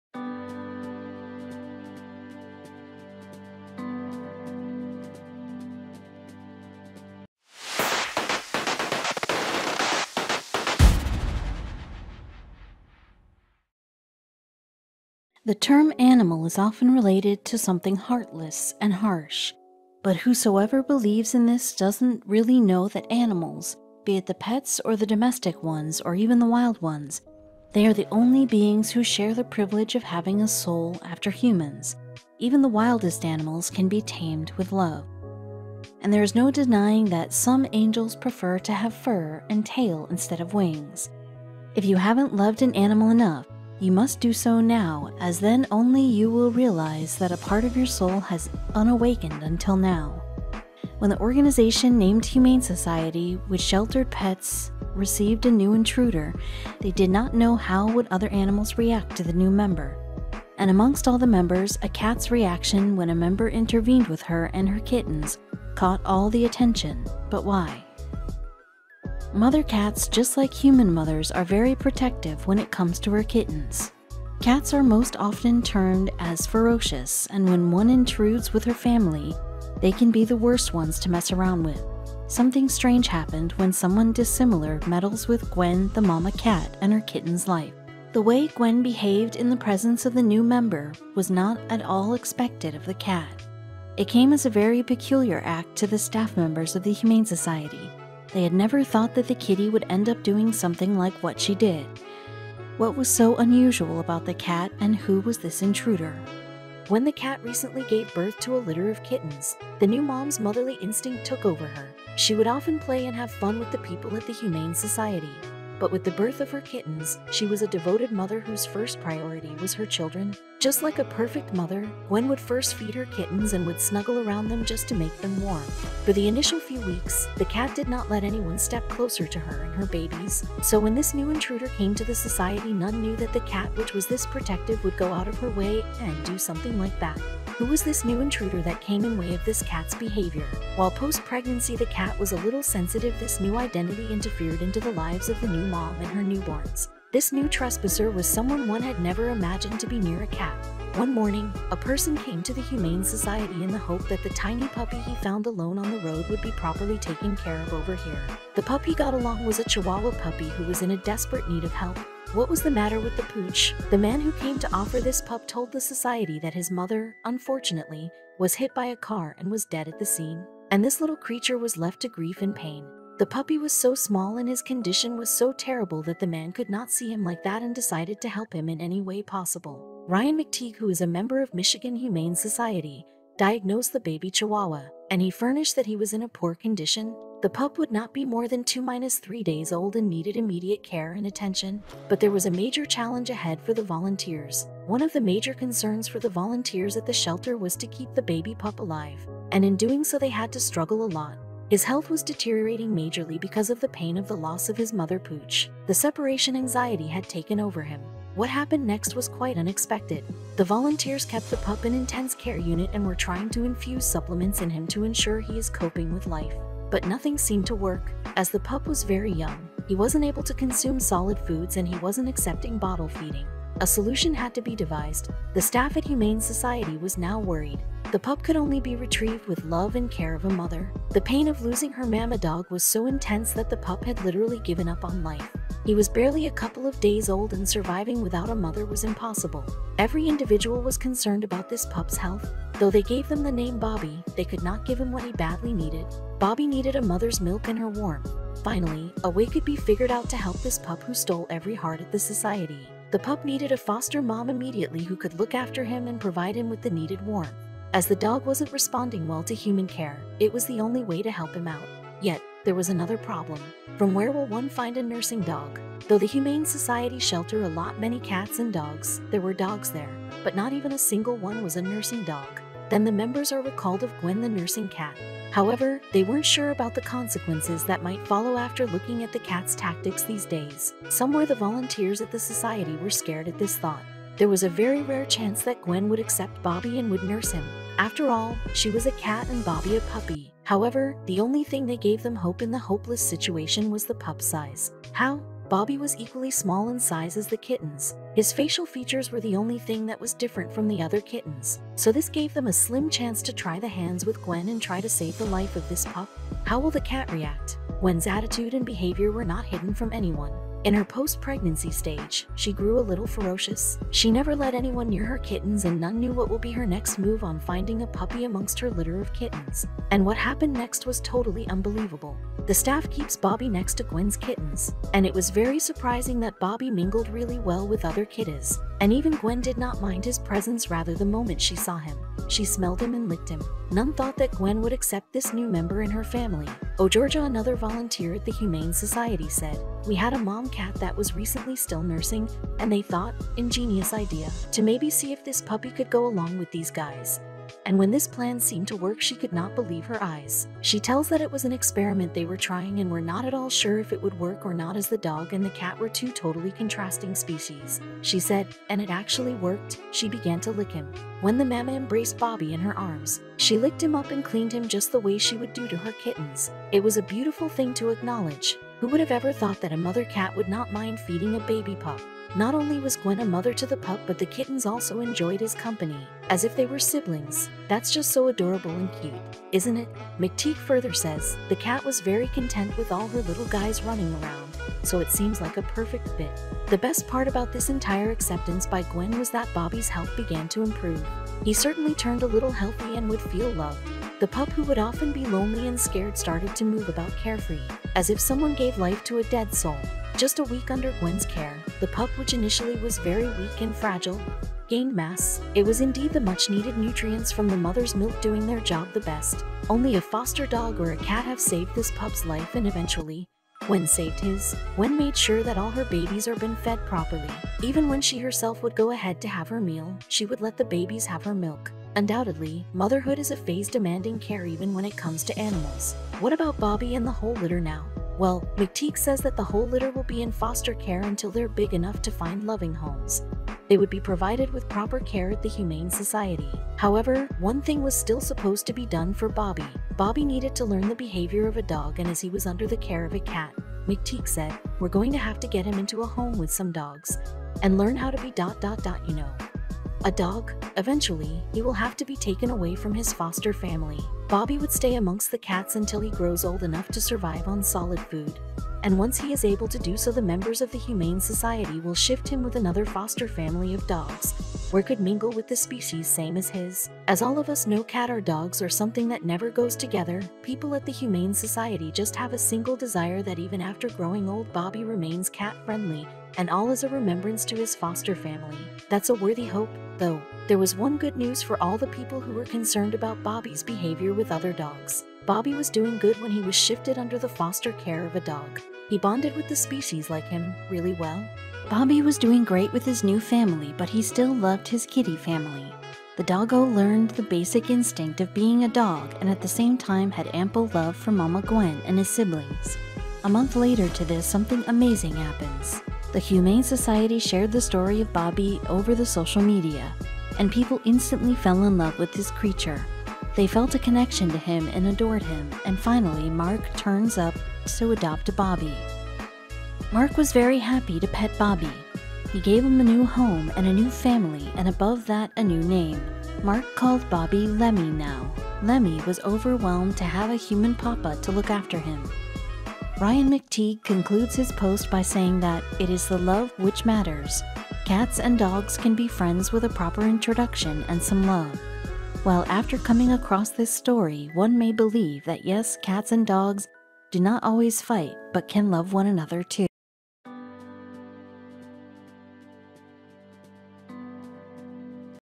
Mamma Cat Won’t Let Anyone Around Her Kittens But A Stranger’s Presence Made Her React Unexpectedly
Popular TikTok